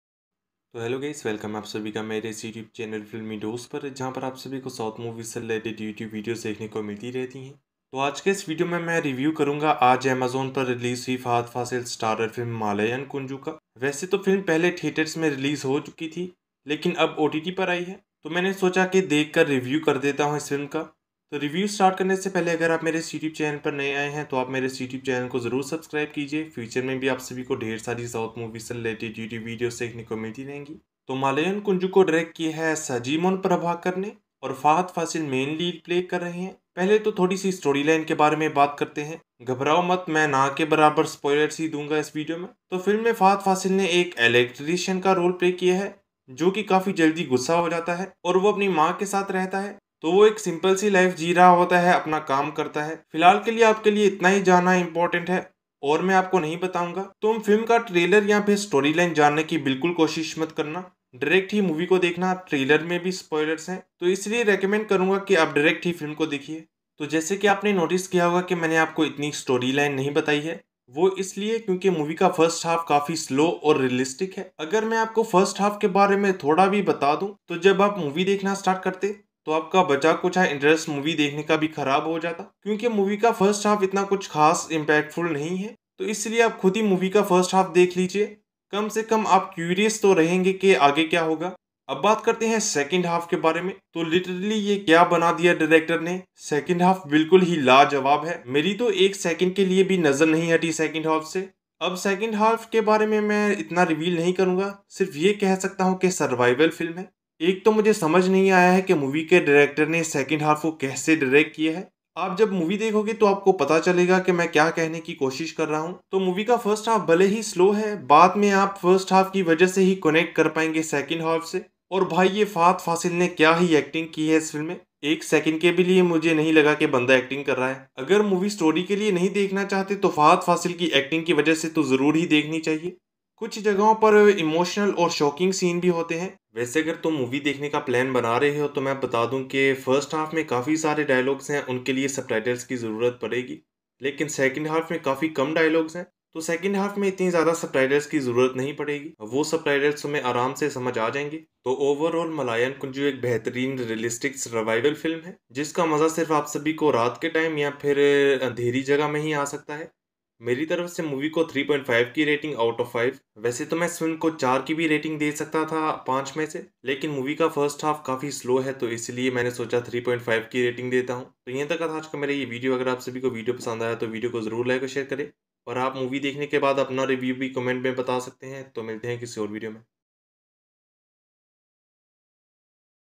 Malayankunju Movie Review Hindi | Fahadh Faasil | Malayan Kunju Review In Hindi | Survival Movie 🔥
Hello Guys,
In This Video I Have Given You All Hindi Review Of Fahadh Faasil Starrer Movie  Malayankunju. Hope You All Like Today's Video.

If You All Are Planning To Watch Malayankunju Full Movie. Then You Guys Definetly Should Watch My Video Till End ♥️🥰

About Malayankunju Movie: Malayankunju is a 2022 Indian Malayalam-language survival thriller film directed by Sajimon Prabhakar and written by Mahesh Narayanan. It stars Fahadh Faasil and Rajisha Vijayan in the lead roles and features Indrans, Jaffar Idukki and Deepak Parambol in supporting roles. Along with the script Mahesh Narayanan handles the cinematography and Arju Benn edits the film. The music of the film is composed by A. R. Rahman.